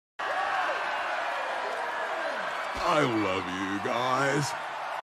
My Reaction That "Chip 'n Dale: Rescue Rangers" Is Launching on Disney+ This Month
This is great news!!! "Chip 'n Dale: Rescue Rangers" is launching on Disney+ this month.